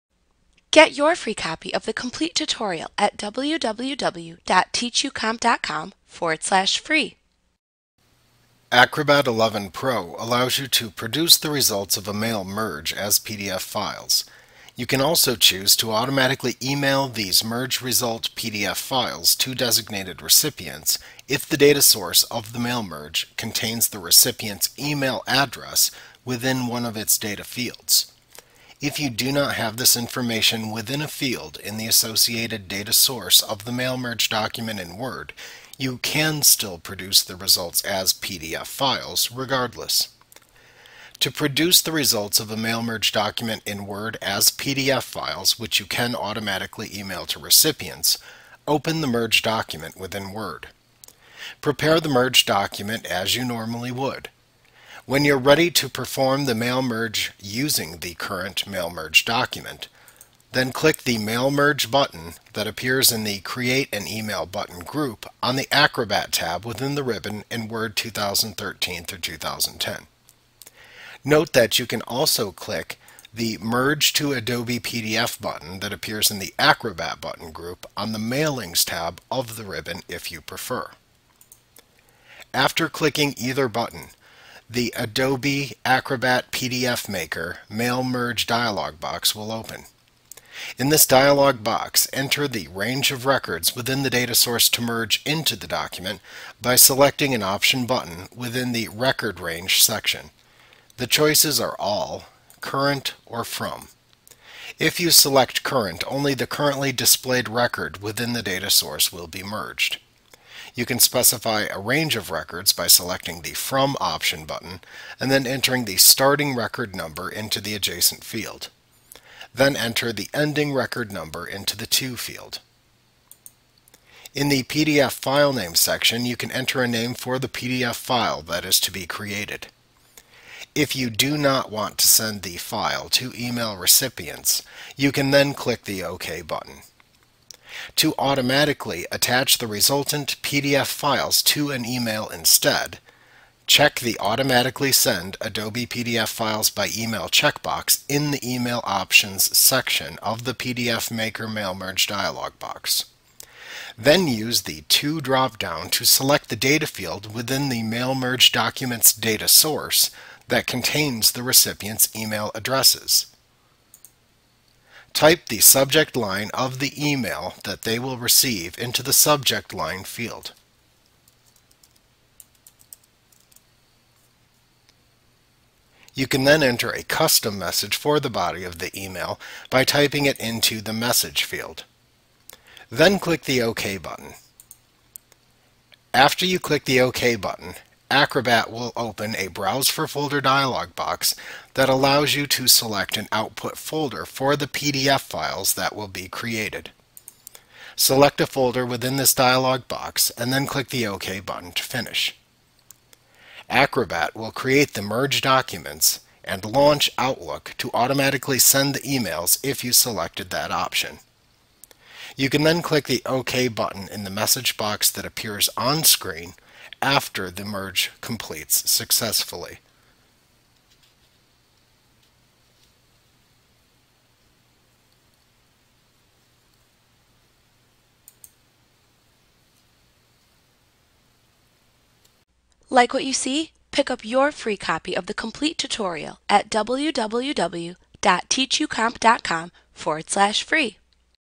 Mail Merge and Email in Word - Adobe Acrobat XI Training Tutorial Course
Learn about Mail Merge and Email in Word in Adobe Acrobat XI Visit us today!